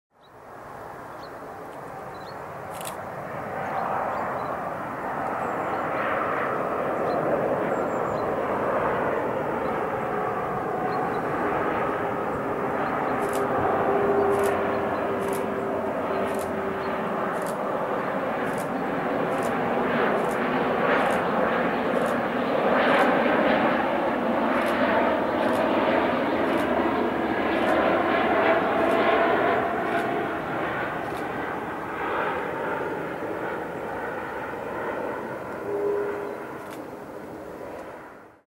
XL Airways France Airbus A330 F-GRSQ short approach Hamburg Airport - 02.12.2012
Kam aus Paris (CDG) und ging zur Lufthansa Technik.

Arriving from Paris (CDG) for maintenance at Lufthansa Technik.